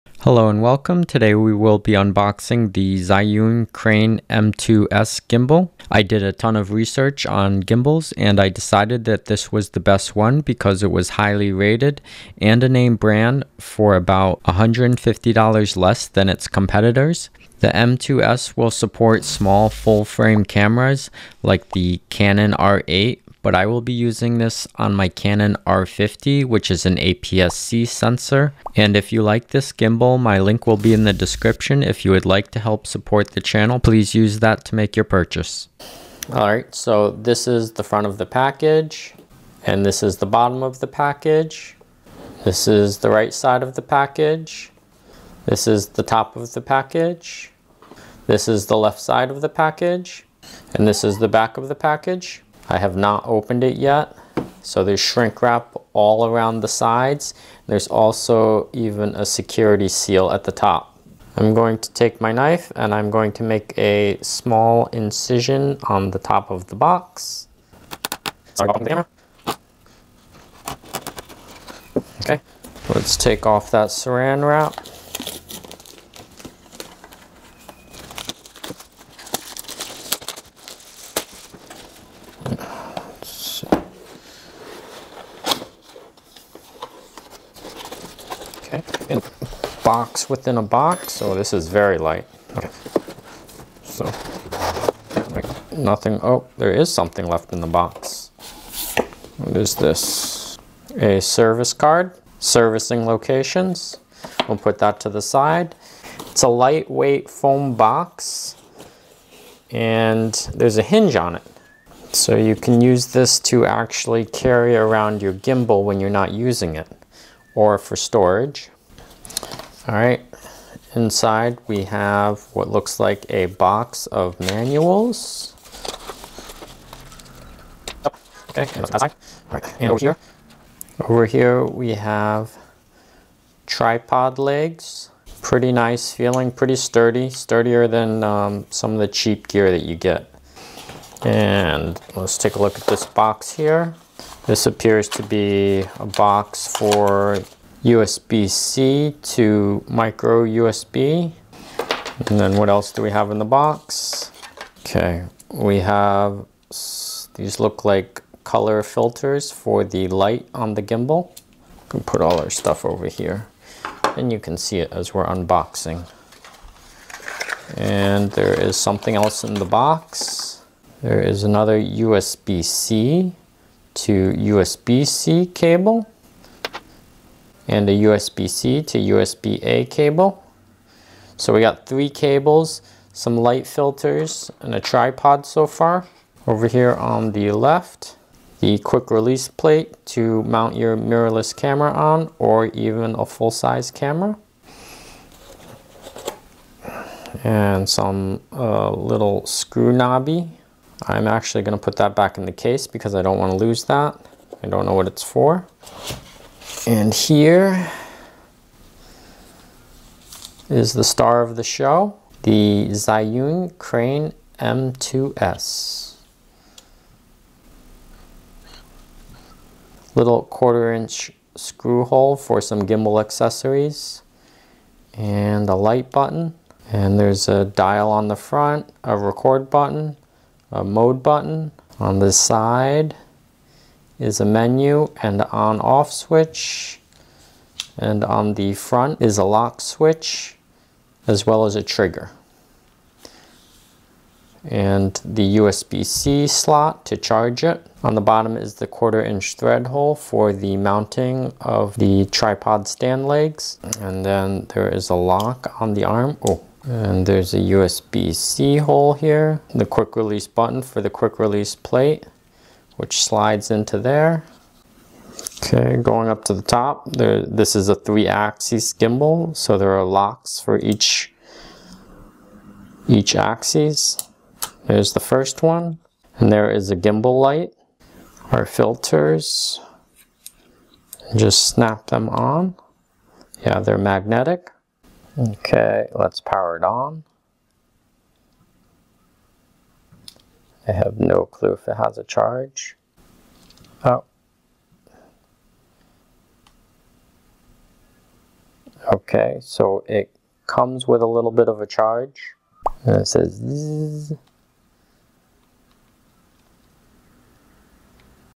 Zhiyun Crane M2S Unboxing | Best Budget Mirrorless Gimbal
What comes in the gimbal box. This is not the combo that includes a mirrorless camera backpack and phone holder.  This is the most basic package that only includes whatever if usually packaged with the Zhiyun Crane M2S.

Lets unbox the Zhiyun Crane M2s and take a look at all the buttons and accessories that are included in the boxed package. Keep in mind that there is a crane combo available that includes a phone mount and backpack. Even if you didn't purchase that package you can get the parts separately.

IMO: This is the best budget mirrorless gimbal for all APS-C Sensor cameras like the Sony ZV-E10, Canon R50, and compact full frame cameras like the Canon R8.

Zhiyun Crane M2S

My Desk Mic
My Audio Interface
My Camera Mic
My Vlog Camera